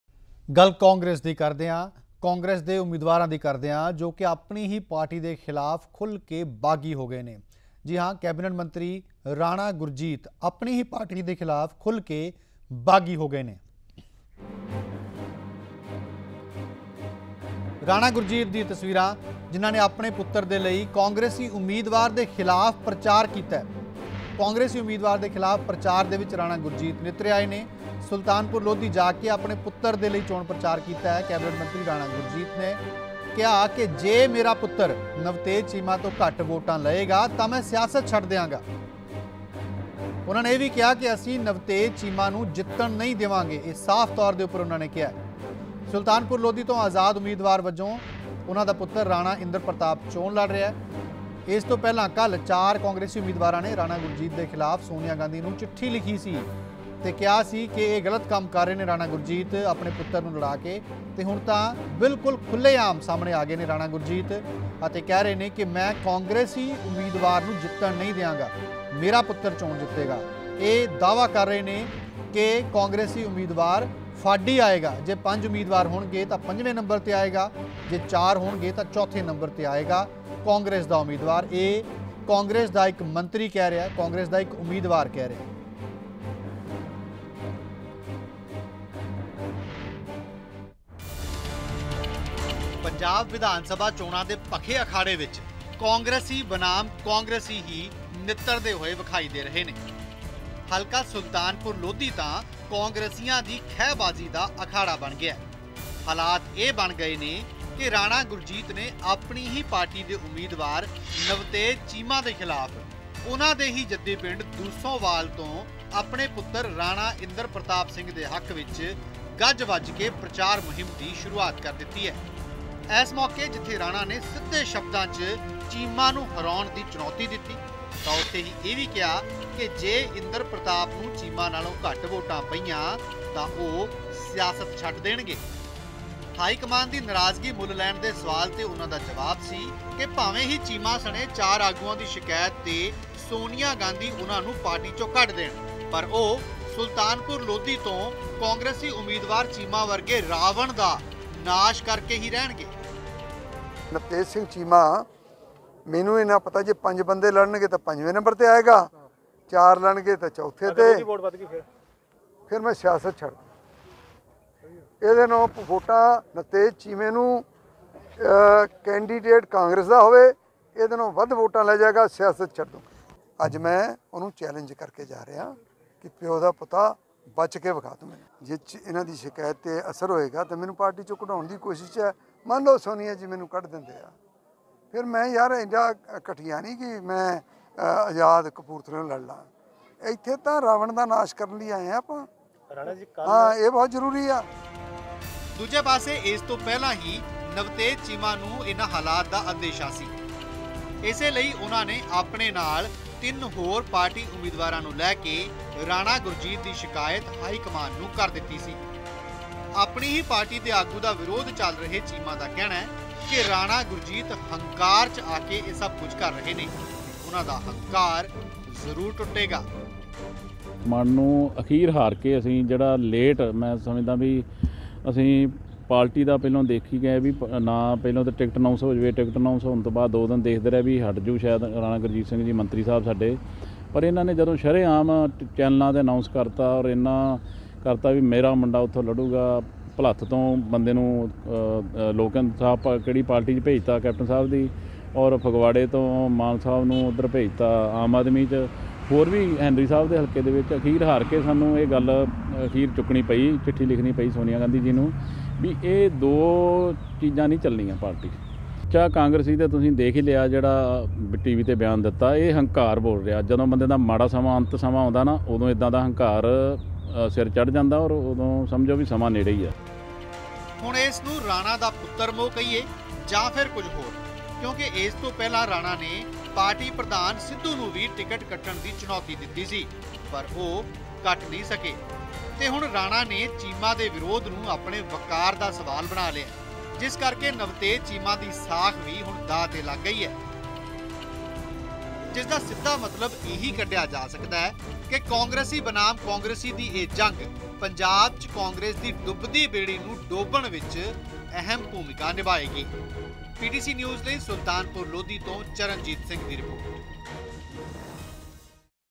ਆਪਣੀ ਹੀ ਪਾਰਟੀ ਖ਼ਿਲਾਫ਼ ਖੁੱਲ੍ਹ ਕੇ ਬਾਗ਼ੀ ਹੋਏ ਕੈਬਨਿਟ ਮੰਤਰੀ ਰਾਣਾ ਗੁਰਜੀਤ
ਆਪਣੇ ਪੁੱਤਰ ਲਈ ਕਾਂਗਰਸੀ ਉਮੀਦਵਾਰ ਖ਼ਿਲਾਫ਼ ਪ੍ਰਚਾਰ 'ਚ ਨਿਤਰੇ ਮੰਤਰੀ ਰਾਣਾ ਗੁਰਜੀਤ। ਆਪਣੀ ਹੀ ਪਾਰਟੀ ਖ਼ਿਲਾਫ਼ ਖੁੱਲ੍ਹ ਕੇ ਬਾਗ਼ੀ ਹੋਏ ਕੈਬਨਿਟ ਮੰਤਰੀ ਰਾਣਾ ਗੁਰਜੀਤ